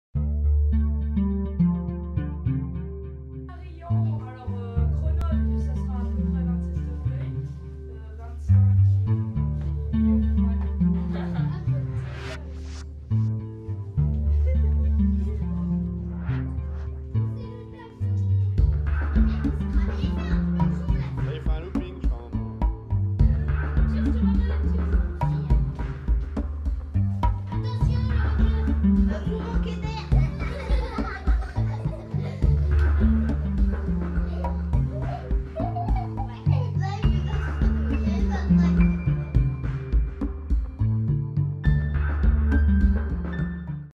Atelier vidéo Isère Savoie / Découverte de l'incrustation
Présentation de l'installation de découverte de l'incrustation sur fond vert.
Chapareillan Octobre 2018.
Incrustation sur fond vert Centre de loisirs de Chapareillan